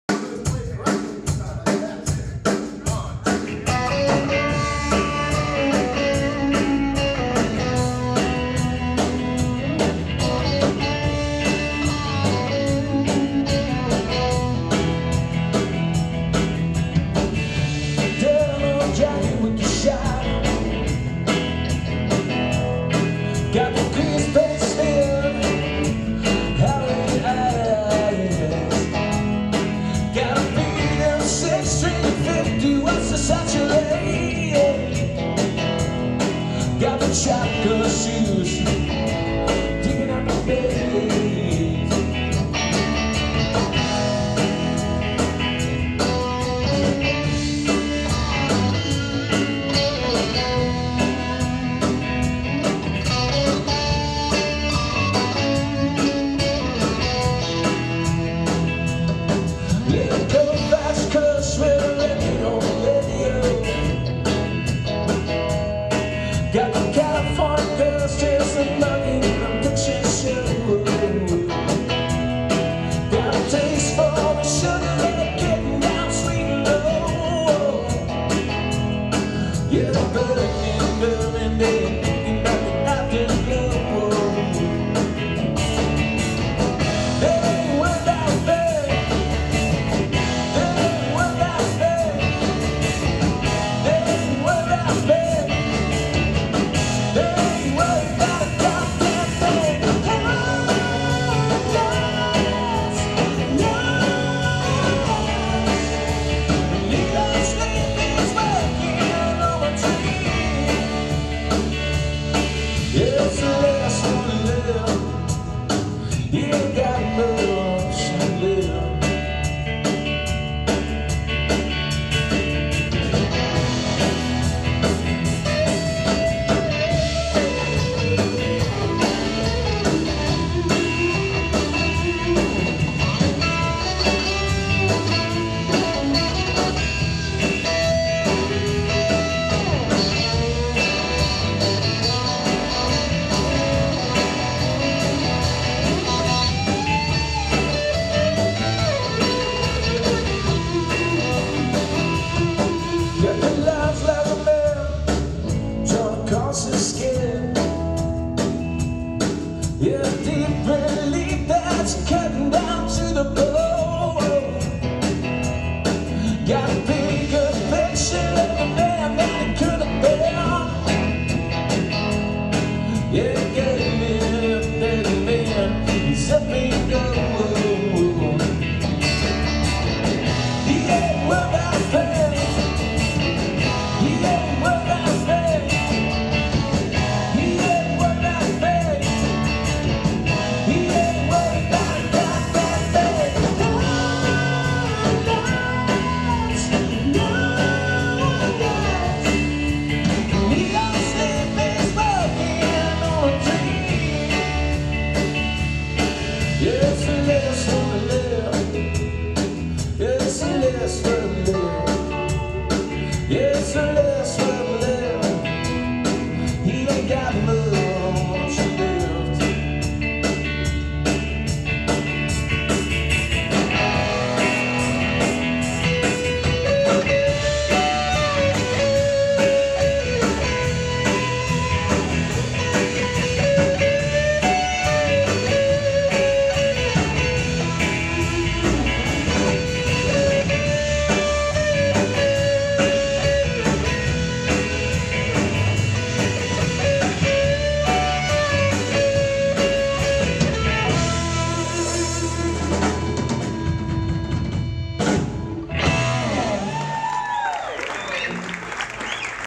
The Noah Gabriel Band performs at The Venue in Aurora
The Noah Gabriel Band performs Aug. 9 at The Venue in Aurora.